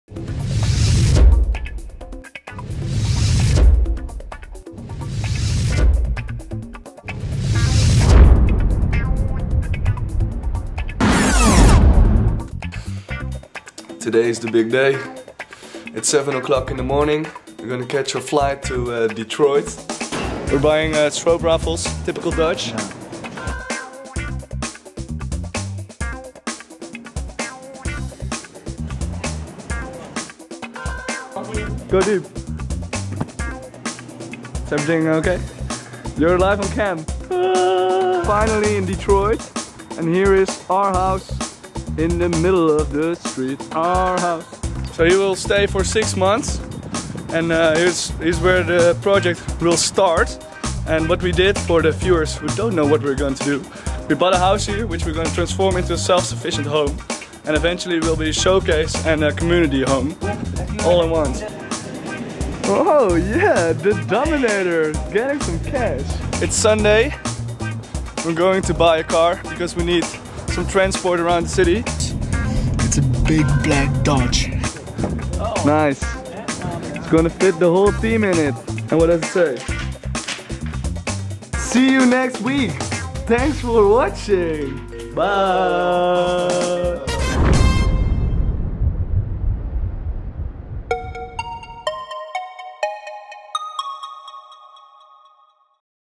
Vlog#1 The Motown Movement
We arrived safely in Detroit! This weekend we handled our jet-leg, arranged some logistic and prepared ourselves for the big steps coming ahead. Check out our first official vlog #1 made by Dutch Picture Industry. Every Monday of the week you will get a look inside the Movement. Hopefully you liked it!